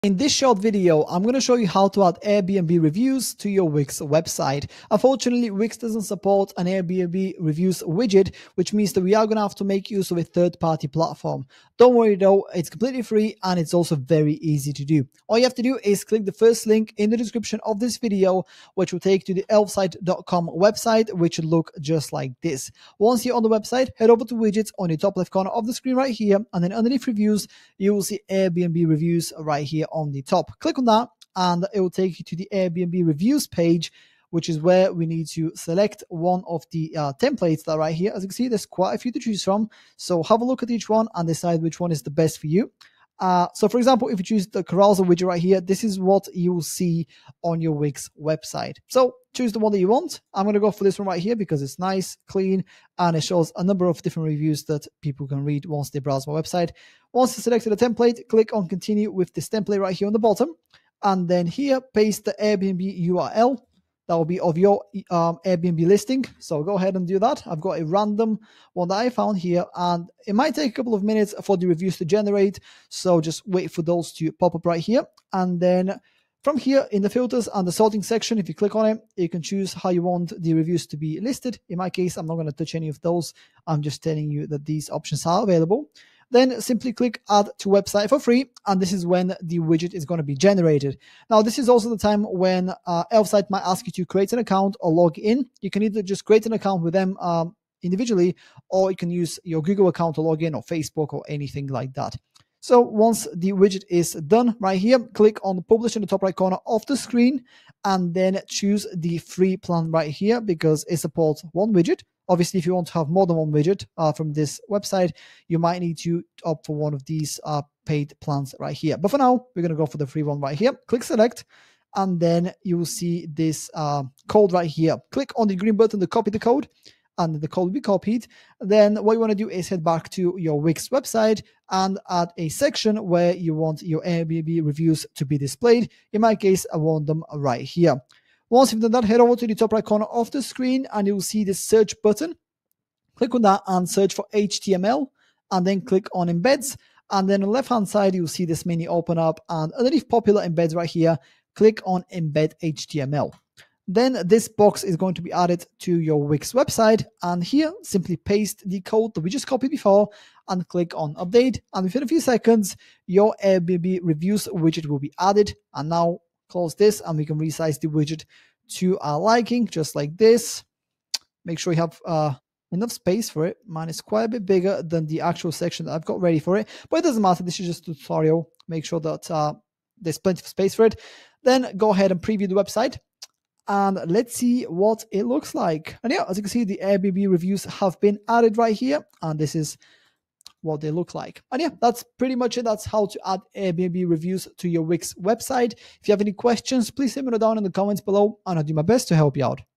How to Add Airbnb Reviews to Wix Website [QUICK GUIDE]
In this video, I'm going to show you how to add Airbnb reviews to Wix website.